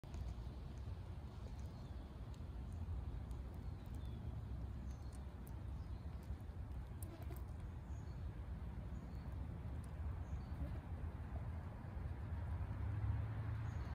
Bicyrtes quadrifasciatus Female Excavates Her Nest
I was photographing some wasps on goldenrod when this female buzzed around my feet. I then realized I was in her way. This short video shows this sand wasp excavating her nest. She has long rakes on her forelegs that she uses to sweep the excavated sand from the burrow.